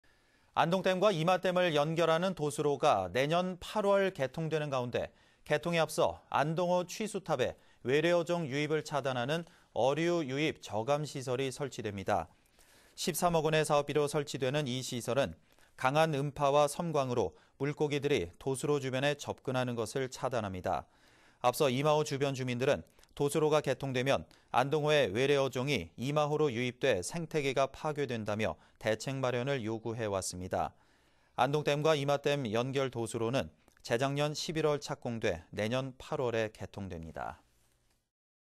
[안동MBC뉴스]외래어종 유입 저감시설 설치
2013/12/18 14:36:37    작성자 : 정윤호

 안동댐과 임하댐을 연결하는 도수로가
내년 8월 개통되는 가운데, 개통에 앞서
안동호 취수탑에, 외래어종 유입을 차단하는
어류유입 저감시설이 설치됩니다.
           ◀ＥＮＤ▶

 13억원의 사업비로 설치되는 이 시설은
강한 음파와 섬광으로 물고기들이
도수로 주변에 접근하는 것을 차단합니다.

 앞서, 임하호 주변 주민들은,
도수로가 개통되면 안동호의 외래어종이
임하호로 유입돼 생태계가 파괴된다며
대책마련을 요구해 왔습니다.

 안동댐과 임하댐 연결 도수로는
재작년 11월 착공돼 내년 8월 개통됩니다